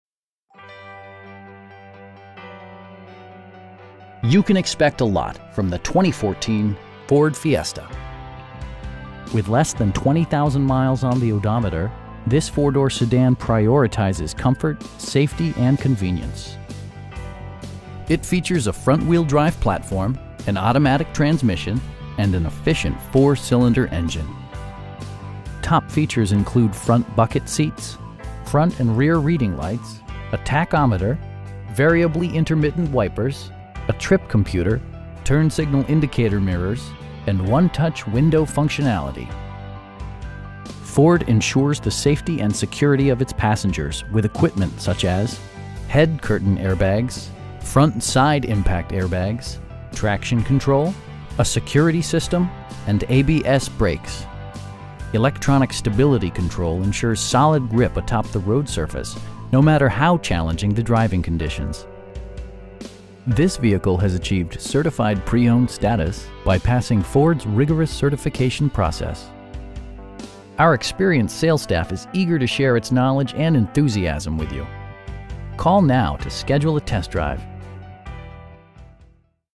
2014 Ford Fiesta SE in Lancaster, CA 93534
You can expect a lot from the 2014 Ford Fiesta. With less than 20,000 miles on the odometer, this 4 door sedan prioritizes comfort, safety and convenience. It features a front-wheel-drive platform, an automatic transmission, and an efficient 4 cylinder engine. Top features include front bucket seats, front and rear reading lights, a tachometer, variably intermittent wipers, a trip computer, turn signal indicator mirrors, an overhead console, and 1-touch window functionality. Ford ensures the safety and security of its passengers with equipment such as: head curtain airbags, front SIDE impact airbags, traction control, a security system, and ABS brakes. Electronic stability control ensures solid grip atop the road surface, no matter how challenging the driving conditions. This vehicle has achieved Certified Pre-Owned status, by passing Ford's rigorous certification process. Our experienced sales staff is eager to share its knowledge and enthusiasm with you. We'd be happy to answer any questions that you may have.